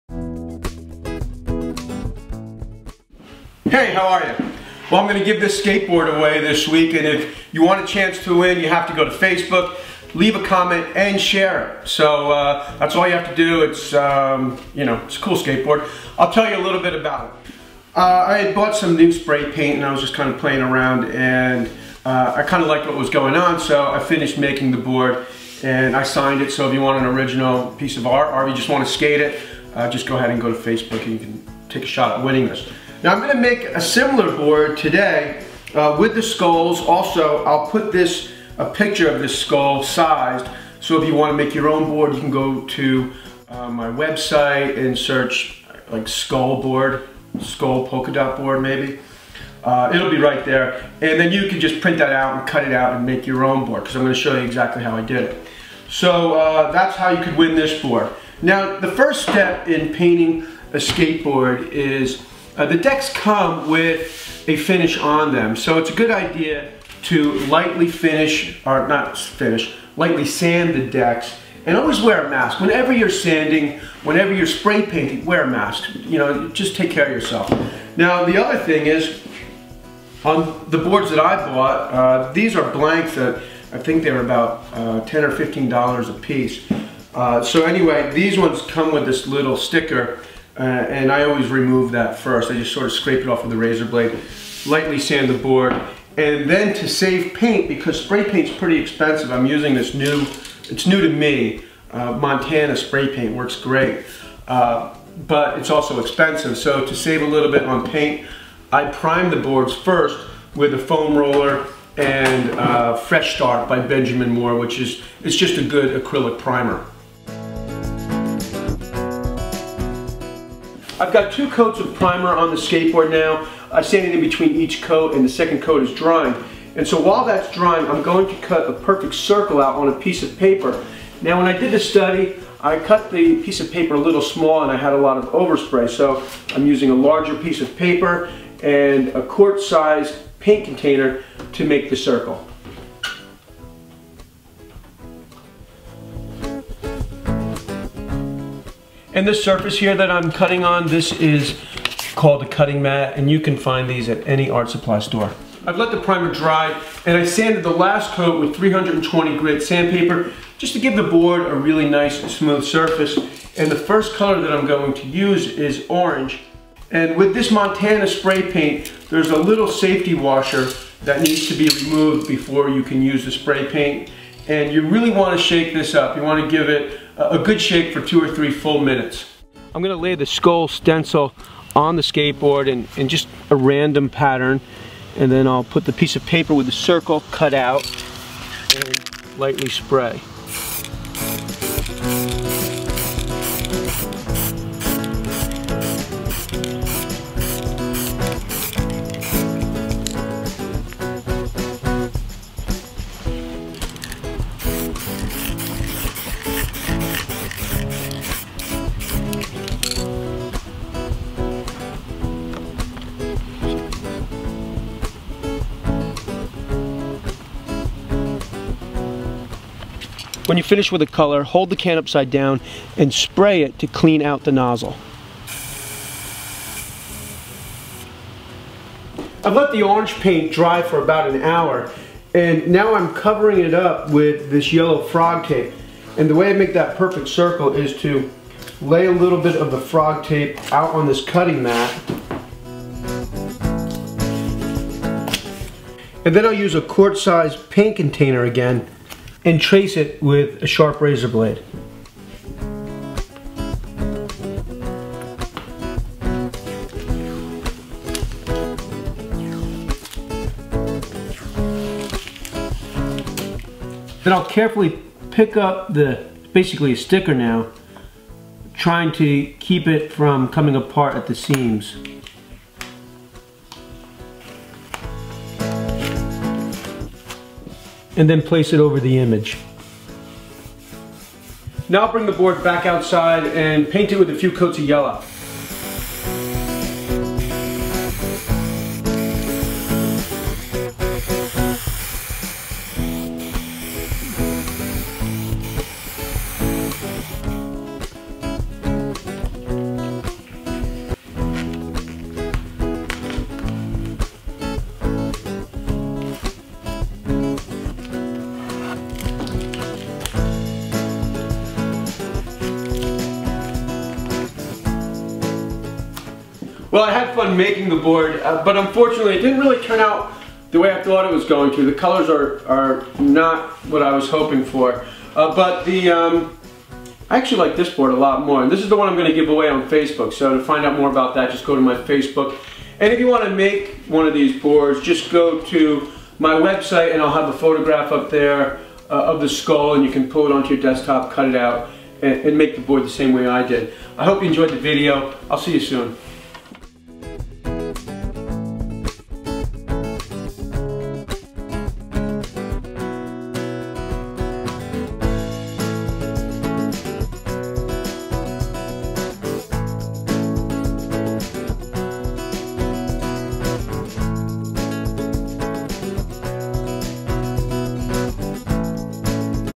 How to Spray Paint a Skateboard with Skull Polka Dots.
Here's a video on making a custom skateboard deck with a skull and polka dot design.  It's easy to do and a really fun project.